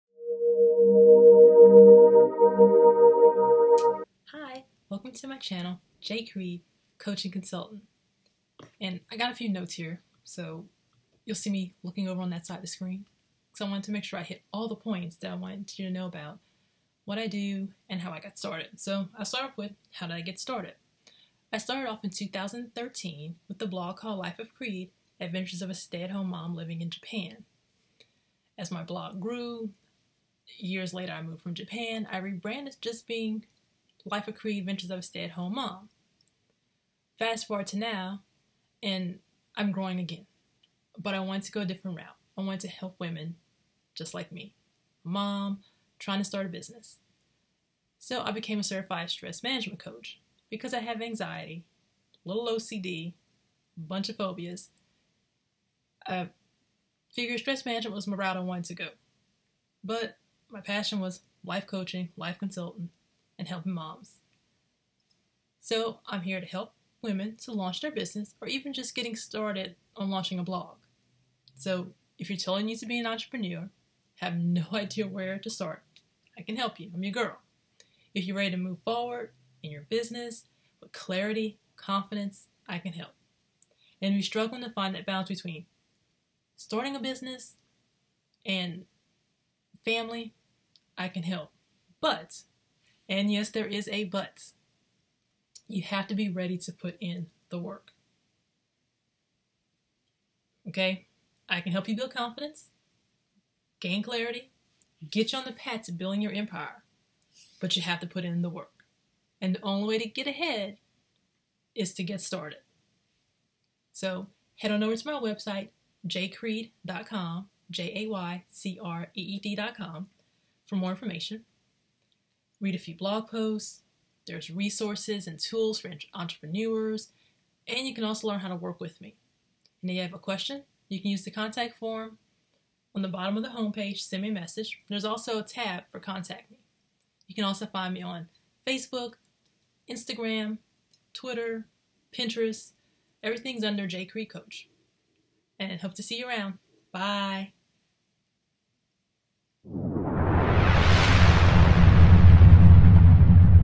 Welcome | Meet Jay, Blog Coach
I'm a Blog Coach and Blogger! I help women go from the thinking about starting a blog to taking action.